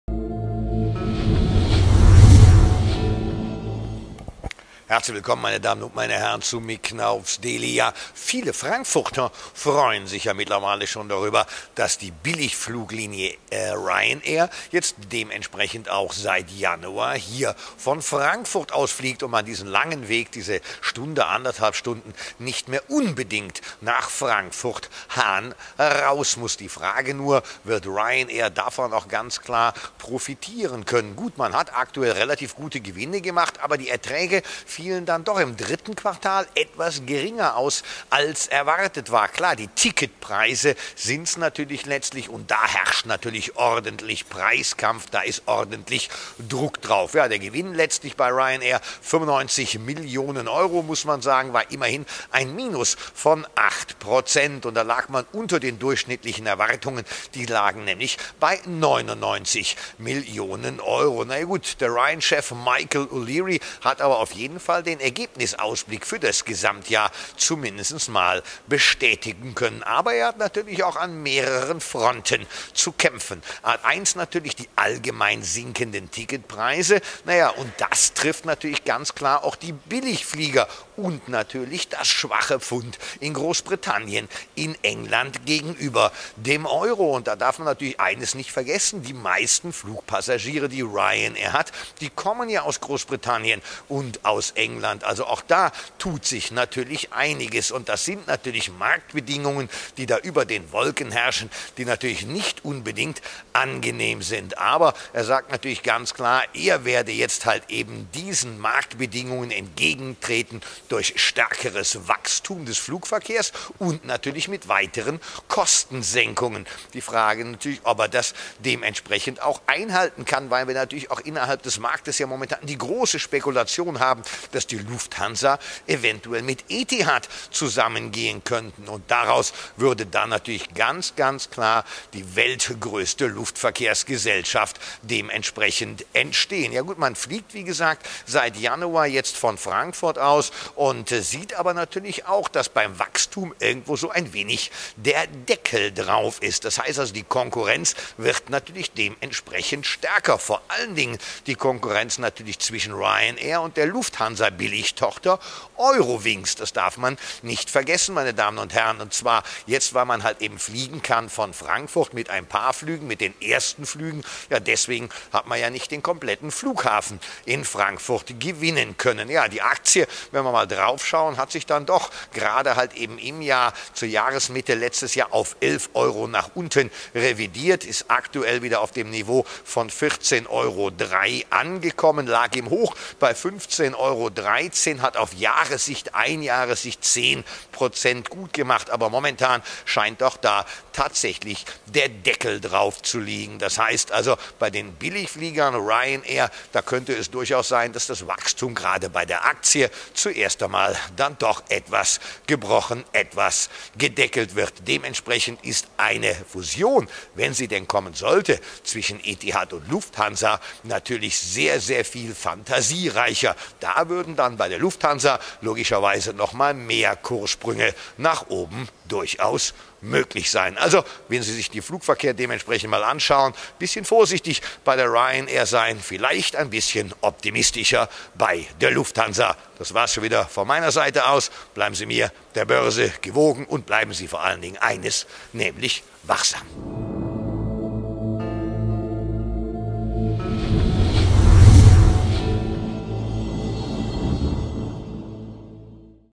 Ryanair enttäuscht – ist Lufthansa jetzt die bessere Airline-Aktie? Mick Knauff's Daily 08.02.2017
Preiskampf bei den Airlines!

Über den Wolken geht es in den letzten Jahren immer härter zur Sache. Sinkende Ticketpreise, heftige Marktbedingungen und auch der Wechselkurs von Pfund zu Euro machen es dabei der Ryanair nicht unbedingt einfacher.